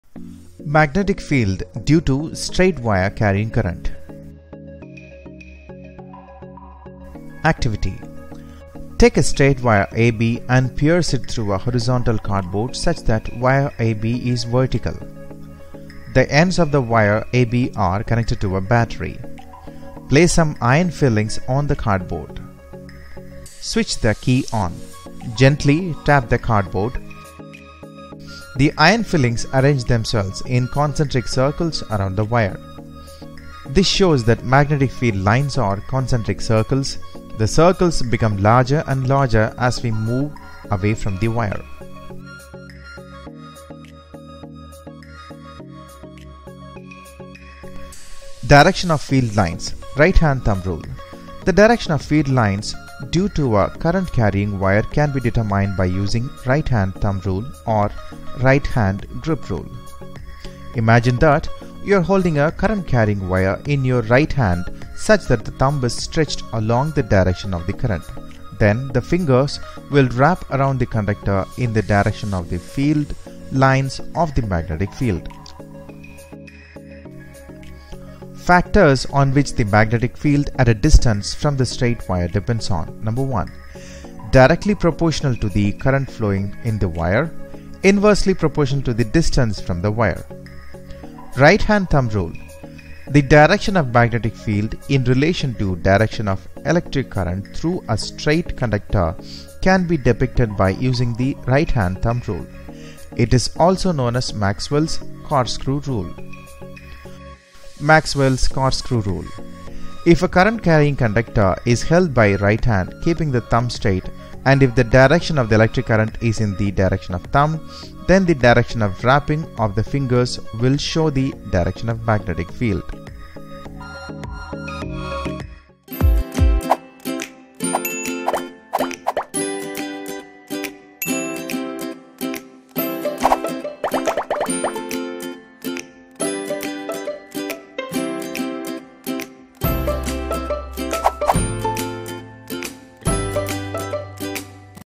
Magnetic Field Due to Straight Wire Carrying Current | Electromagnetism Fundamentals | Physics
Learn about Magnetic field due to straight wire carrying current in electromagnetism fundamentals, physics concepts.

Team SimplyInfo.net